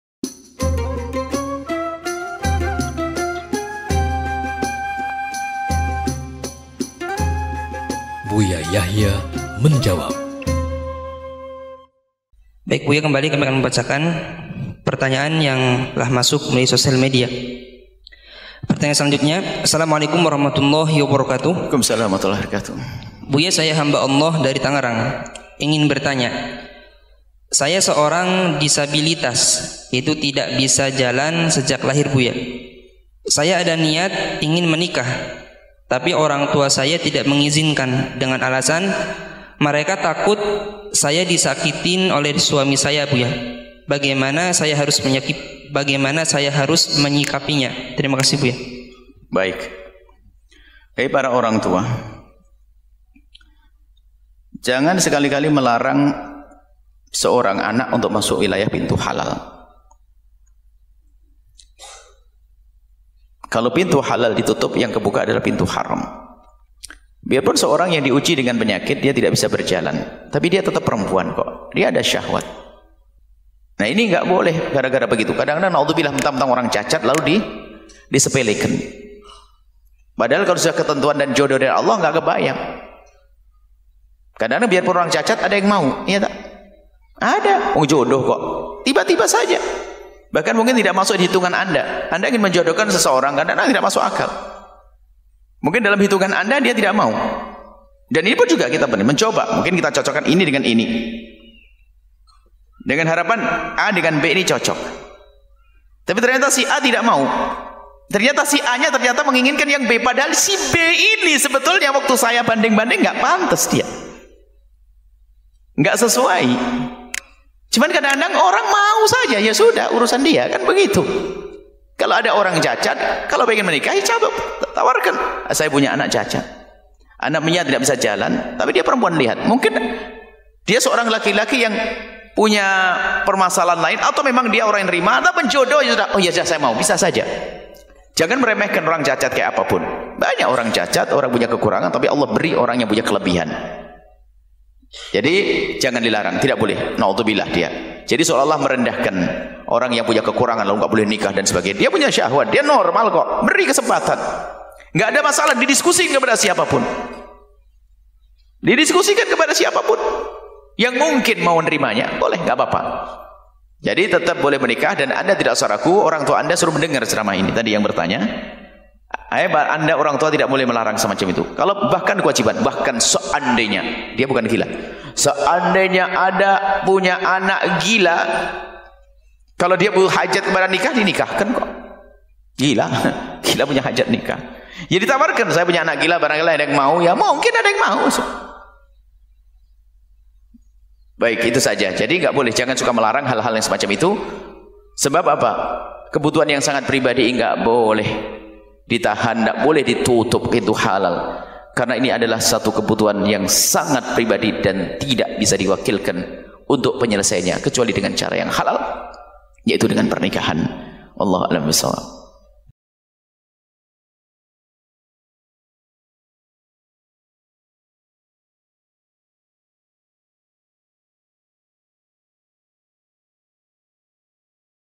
#595 Seorang disabilitas yang ingin menikah namun orang tua tidak mengizinkan | Buya Yahya Menjawab
Seorang disabilitas yang ingin menikah namun orang tua tidak mengizinkan | Buya Yahya Menjawab